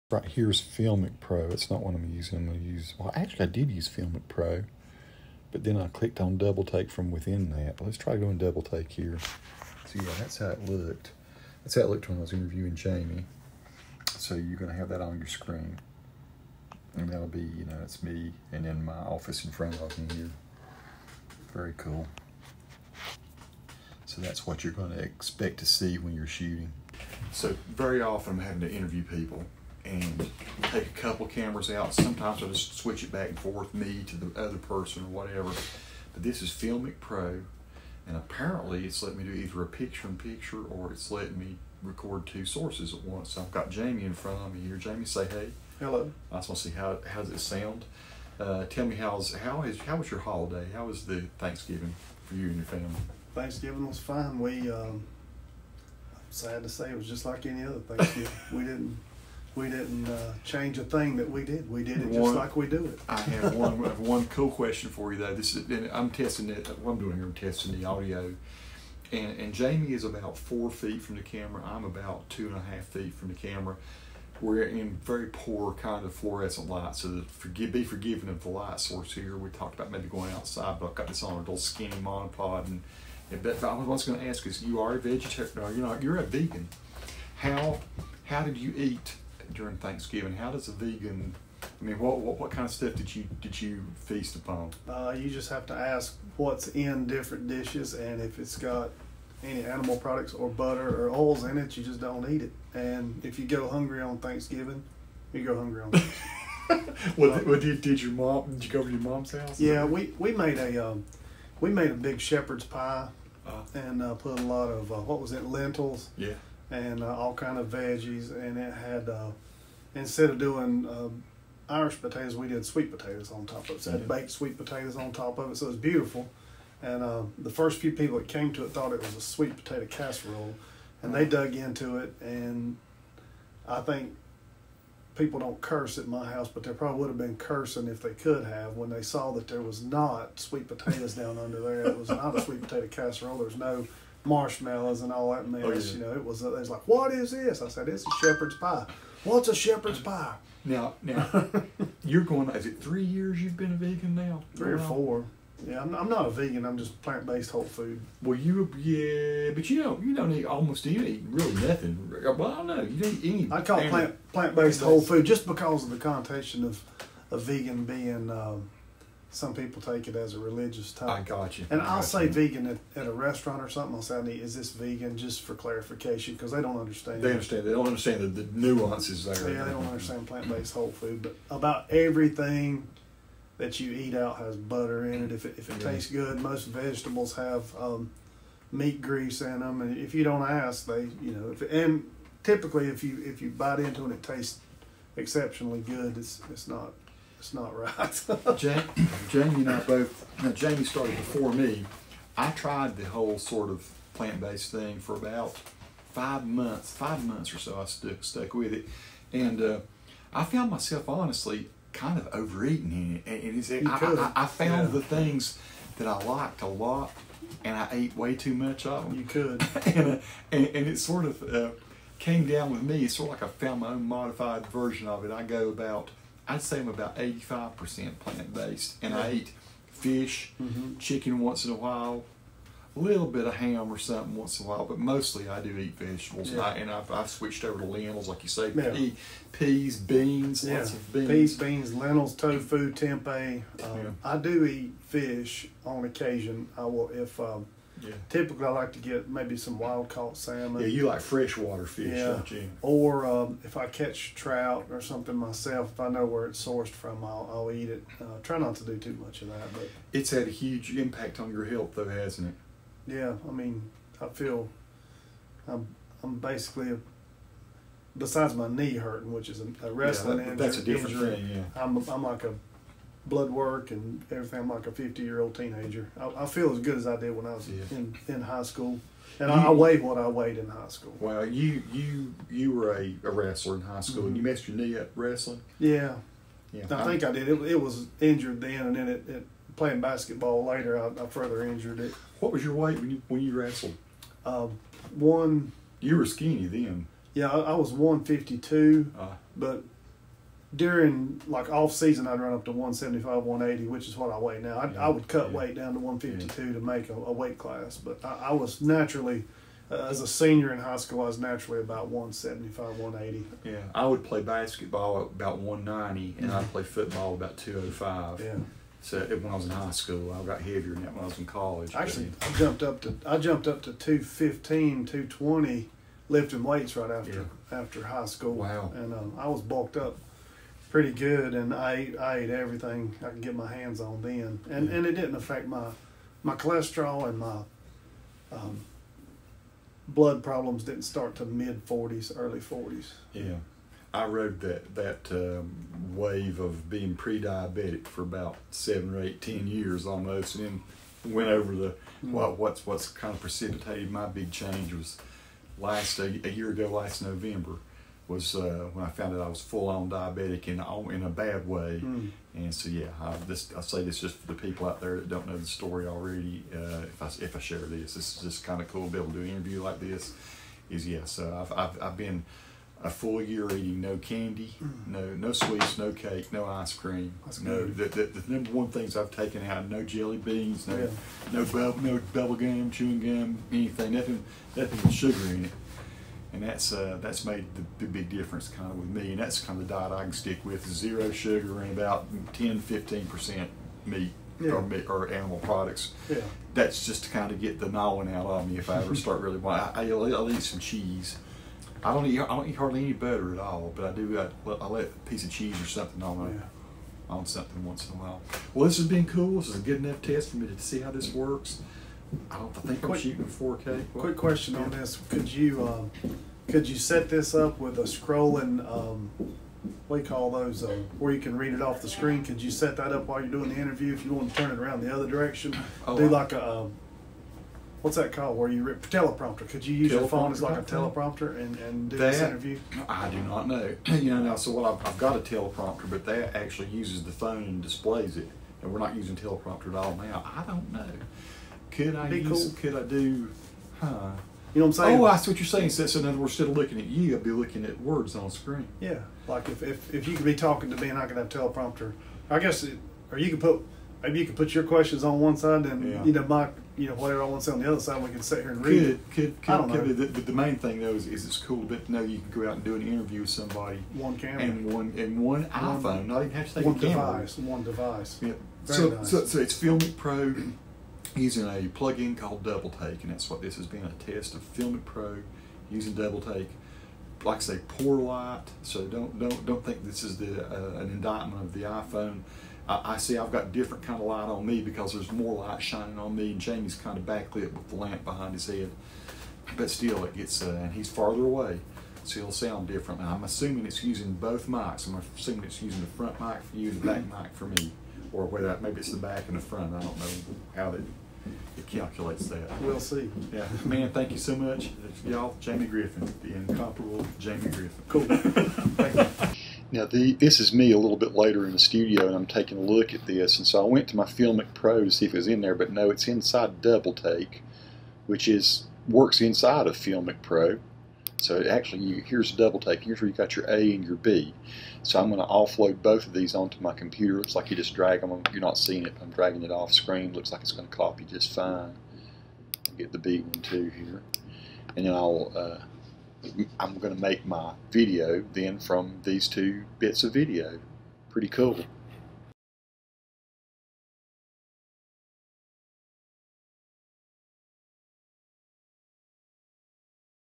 Filmic Pro and DoubleTake Apps on iPhone 12 Pro - Test and Partial Review
This is a test of using a single iPhone 12 Pro to shoot both camera angles of an interview. My friend Jamie Griffin and I conduct a sample interview to test the DoubleTake app that I accessed from a link inside Filmic Pro. I show a few screenshots of how you access the app, and how you can offload the video to your computer for editing. This was shot in a poorly lit environment, but the device still seems to function fairly well. Let me know what you think.